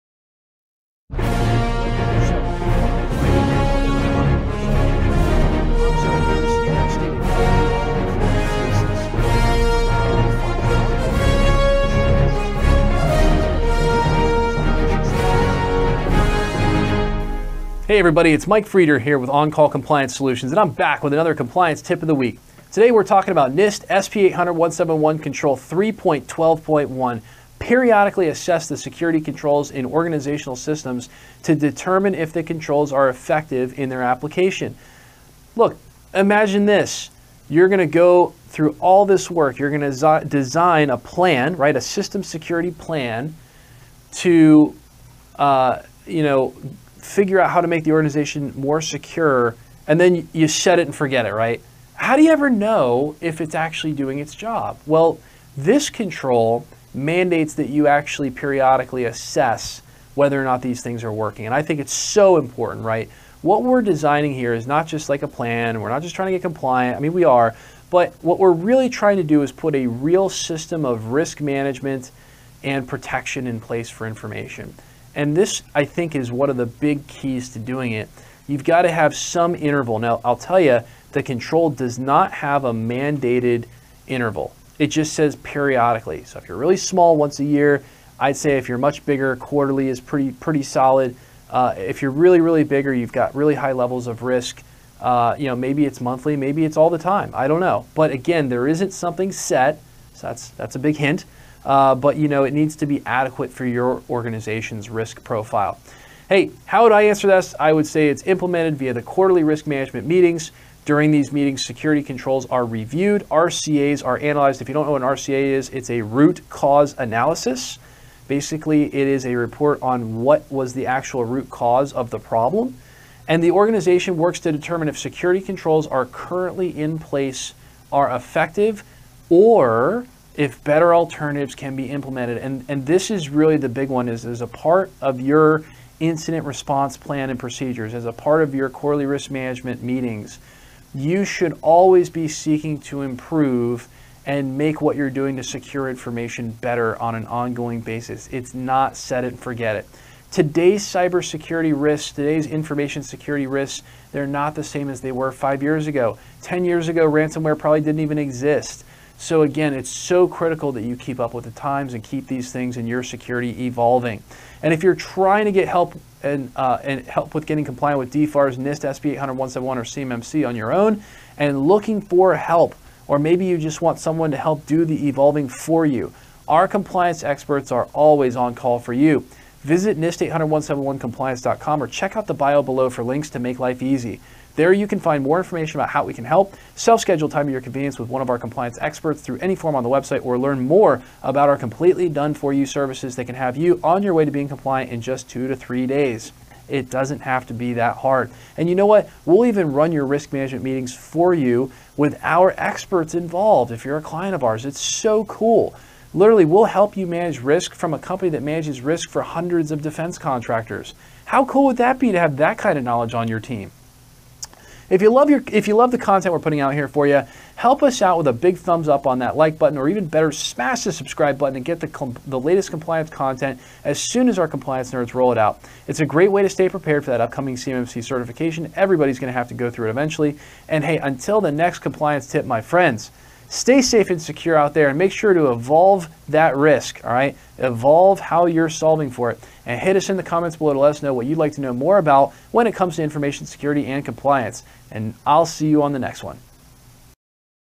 NIST 800-171 Control 3.12.1 –Assess Security Controls to Determine Effectiveness
In this edition of the On Call Compliance Solutions Compliance Tip of the Week, we discuss assessing security controls in organizational systems and the environments in which those systems operate as part of the system development life cycle. By assessing the implemented security controls, organizations determine if the security safeguards or countermeasures are in place and operating as intended. Let’s talk about NIST 800-171 Control 3.12.1 - Periodically assess the security controls in organizational systems to determine if the controls are effective in their application.